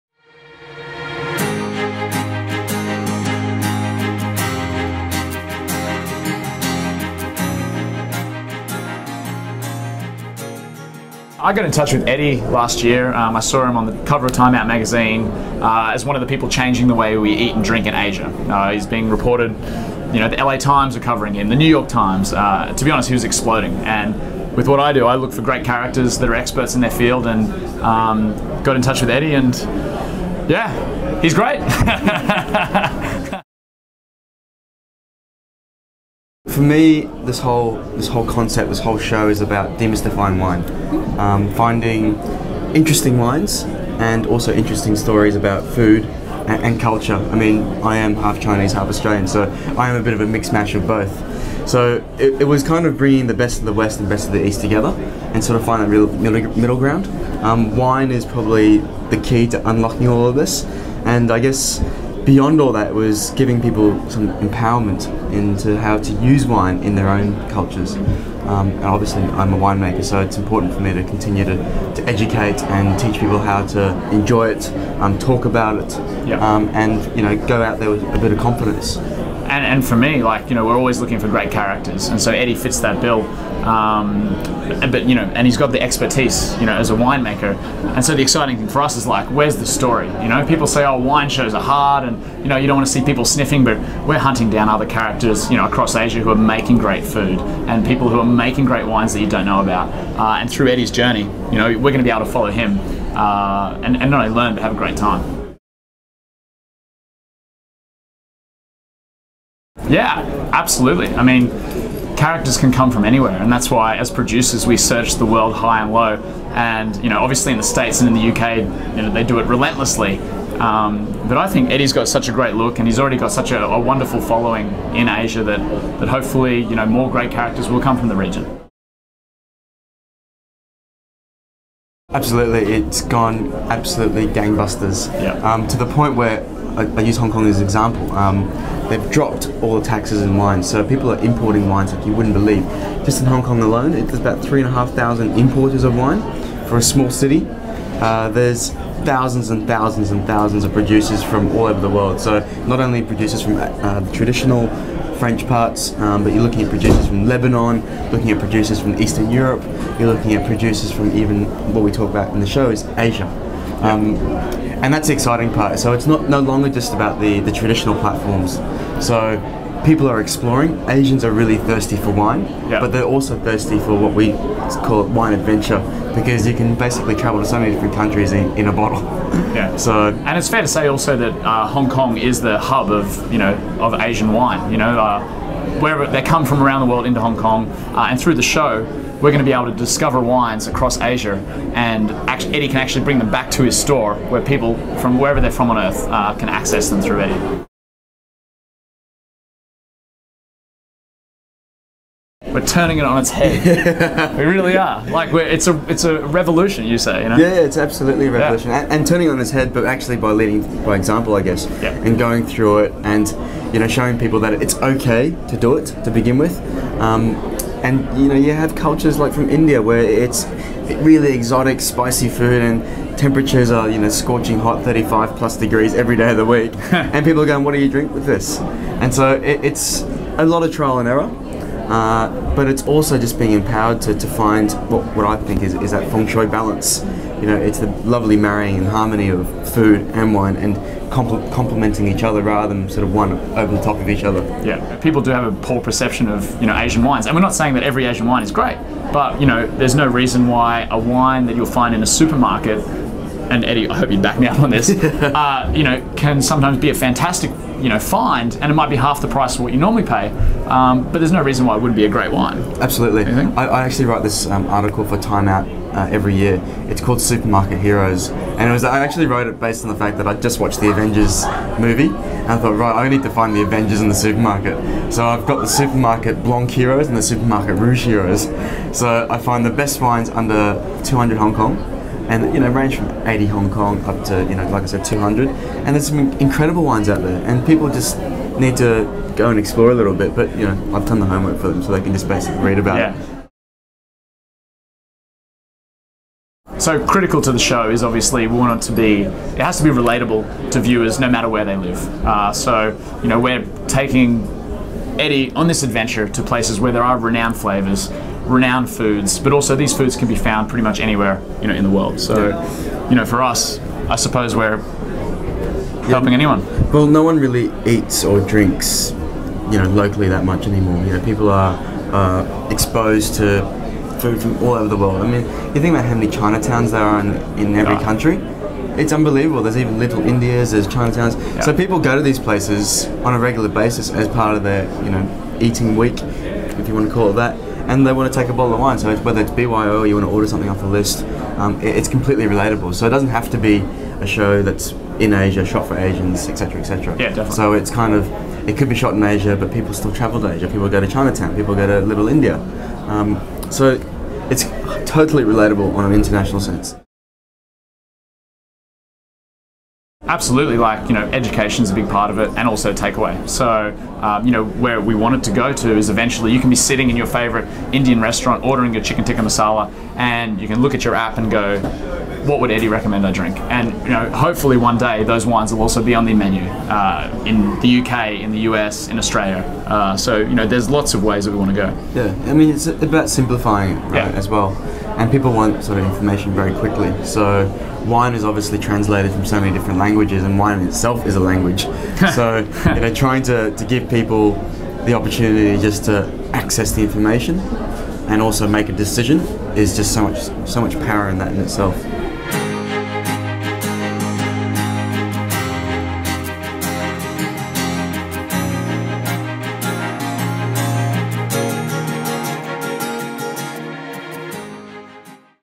The Flying Winemaker's Wes Dening & Eddie McDougall
The owner of a Hong Kong-based boutique wine gallery, Eddie McDougall is on a mission to change the way that people perceive and enjoy Asian food and wine. In The Flying Winemaker, sold by ABC Commercial and produced by WD Entertainment Group, McDougall takes viewers to locations such as China, India, Thailand, Japan and Malaysia to show how Asian meals can by combined with local wines.

In this video interview, McDougall discusses with TV Real Weekly how he is spreading the word about the changing wine culture in Asia through his show. TV Real Weekly also speaks with Wes Dening, the series' executive producer, about the importance of finding strong personalities, like McDougall, to lead programming.